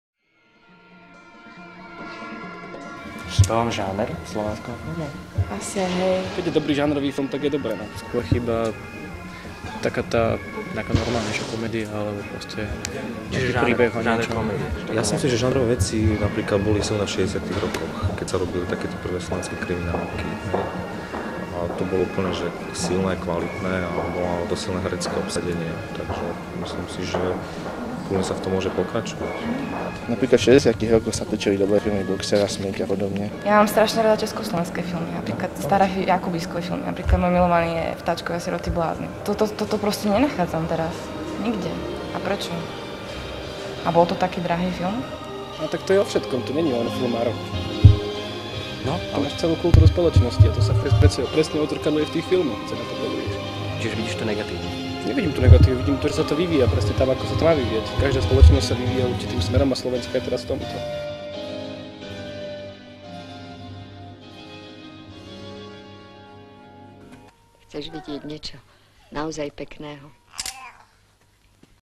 AKÉ NAOZAJ SÚ NOVÉ SLOVENSKÉ FILMY? | 60. roky
Aká je skutočná kvalita slovenského celovečerného filmu vo výnimočne úrodných rokoch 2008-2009? Sú ovácie, či kritika na mieste? Potrebuje domáca kinematografia veľa divákov, alebo skôr elitársku kvalitu? Existuje vôbec na Slovensku kompetentná filmová kritika? Príďte sa aj vy zapojiť do živej diskusie o súčasnej slovenskej kinematografii so zaujímavými hosťami, ktorú povedie šéfredaktor portálu kinema.sk Peter Konečný.